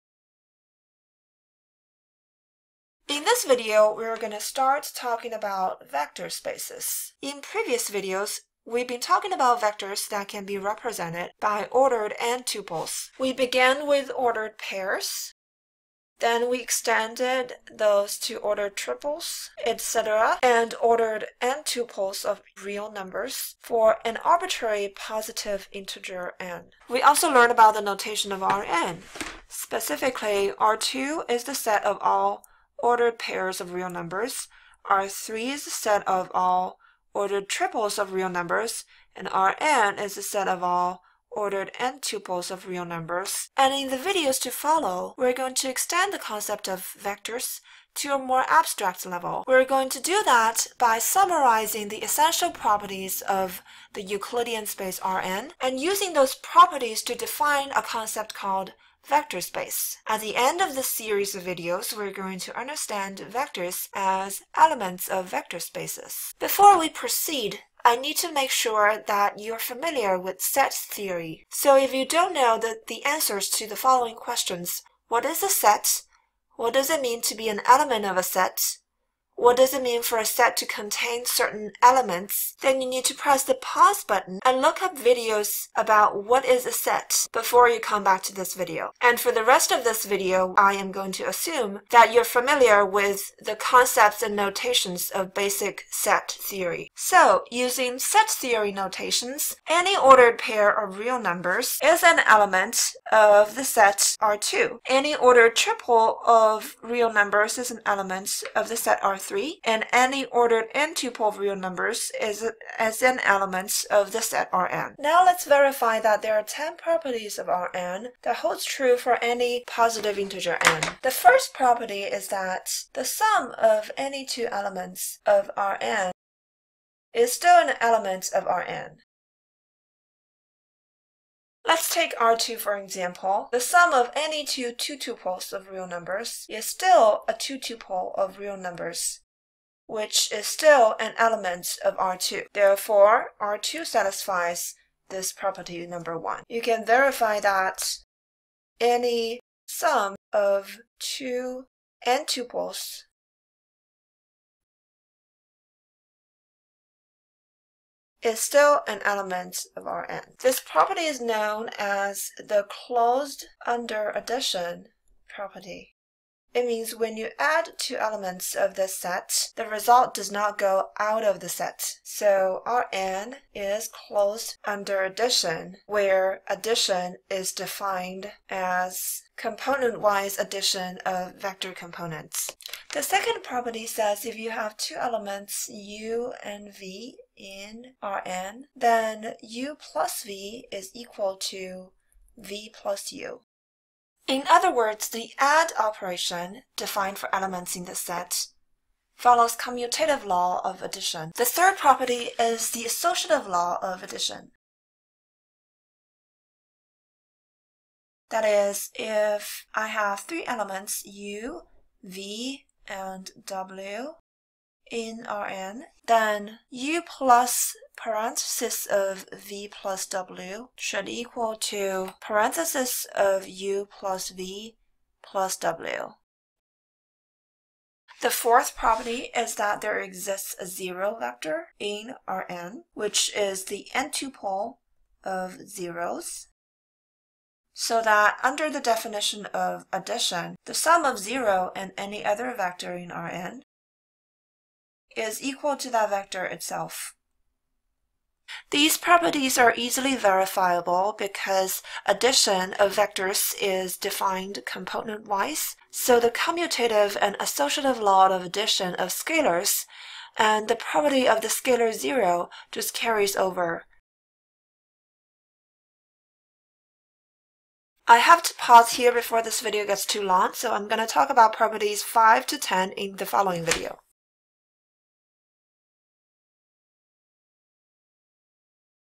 Vector space Part 1: what makes R^n a "space"
This video kicks off the series of videos on vector spaces. We begin by summarizing the essential properties of R^n.